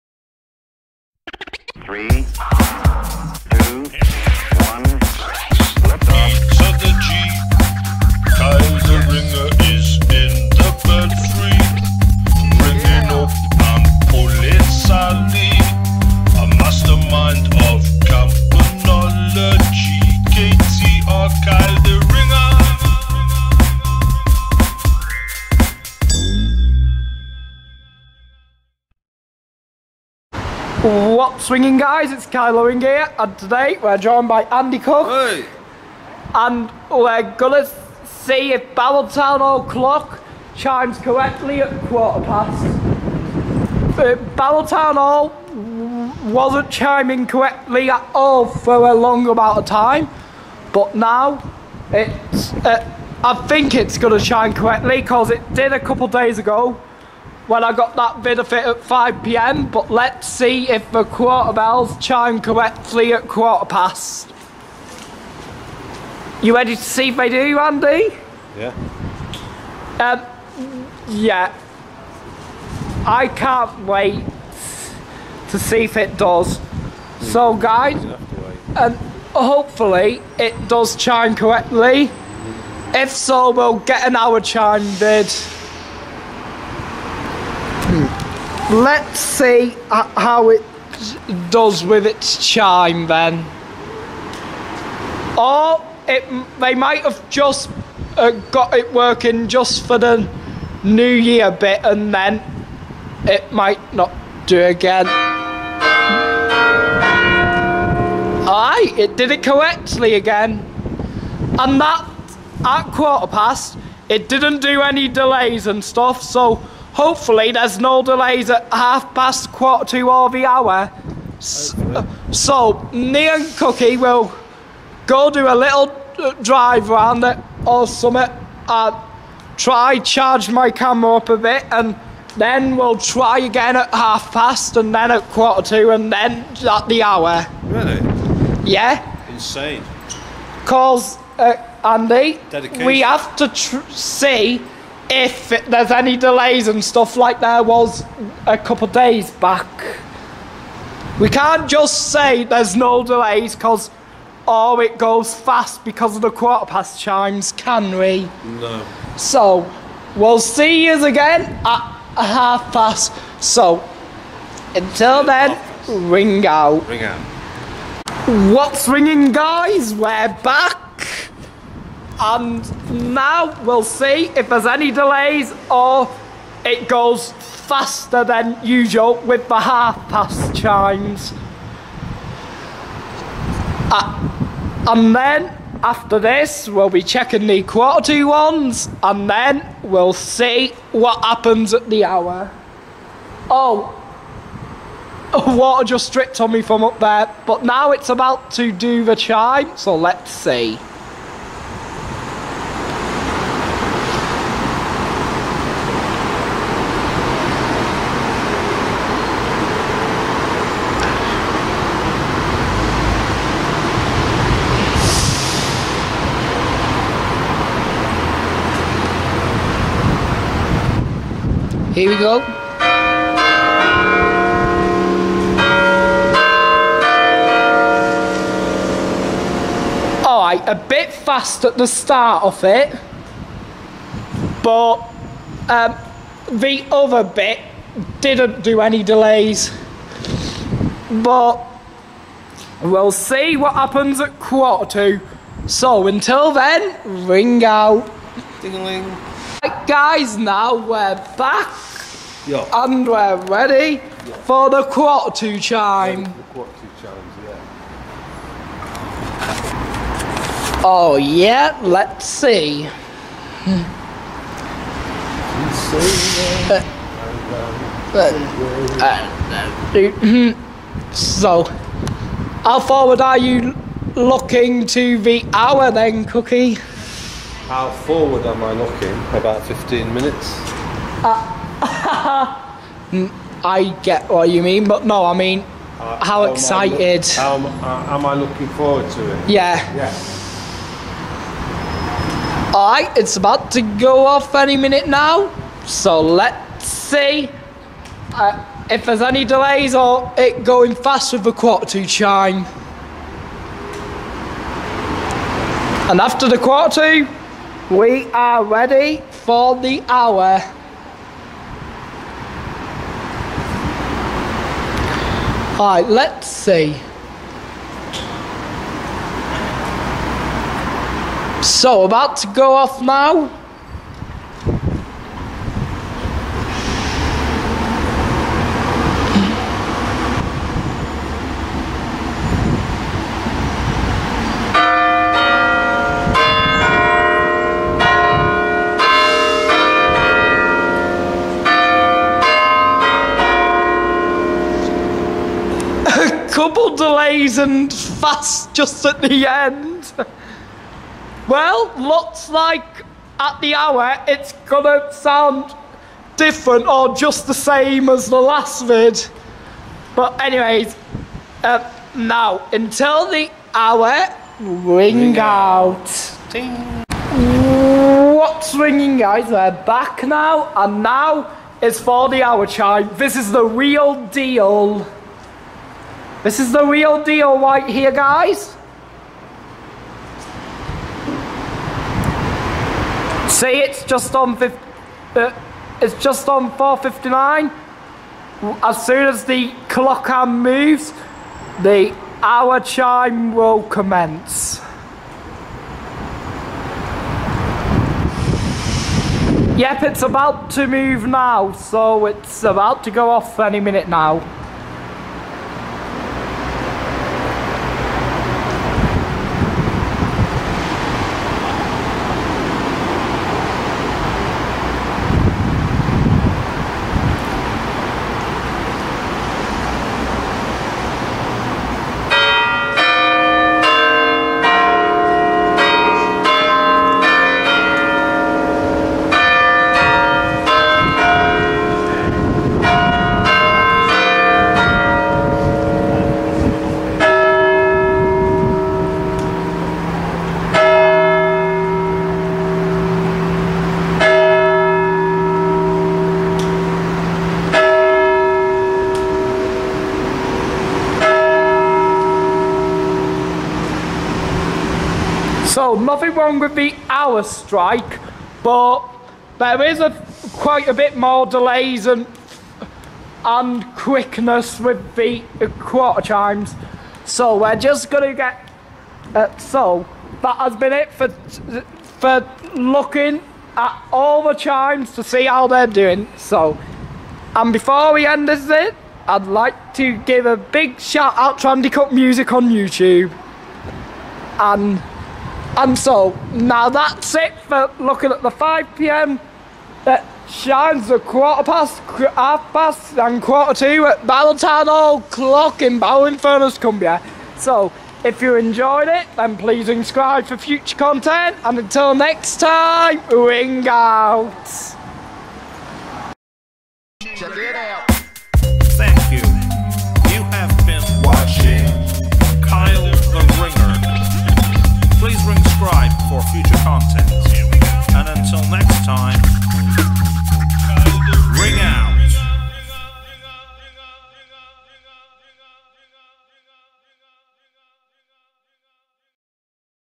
Barrow Town Hall Chimes quarter past half past quarter to and 5pm
0:00 theme song
0:30 Waiting for the Quarter Past Chimes
1:43 The Quarter Past Chimes
2:48 Waiting for the Half Past Chimes
3:40 the Half Past Chimes
4:10 Waiting for the Quarter To Chimes
5:33 The Quarter To Chimes
6:07 Waiting for the 5PM Chimes
7:00 The 5PM Chimes
8:26 ending song